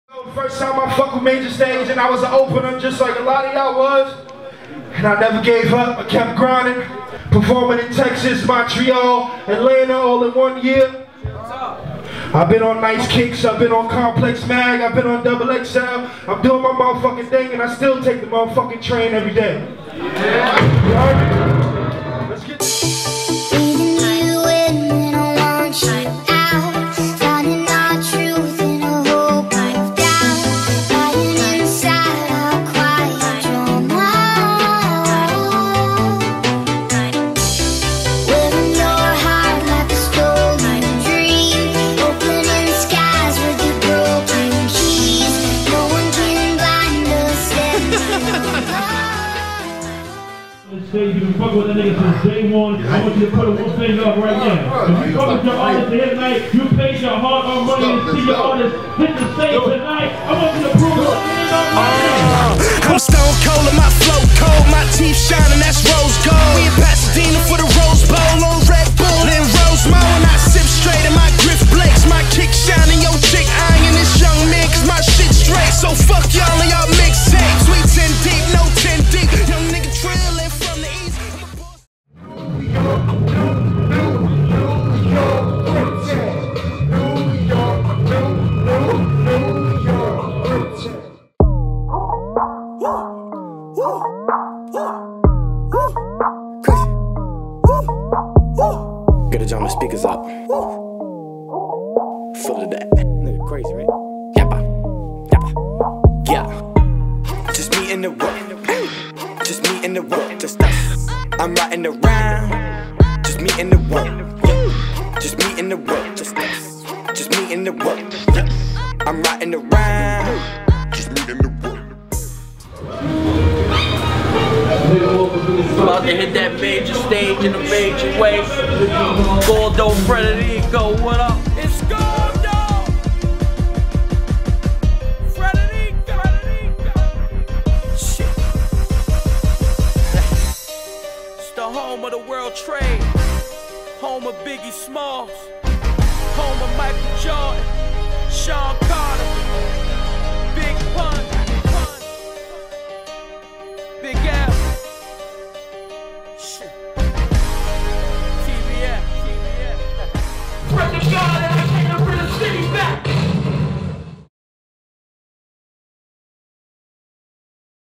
DocuStage series featuring Fred The Godson
New DocuStage series featuring Fred The Godson and more from our New Years Day Concert.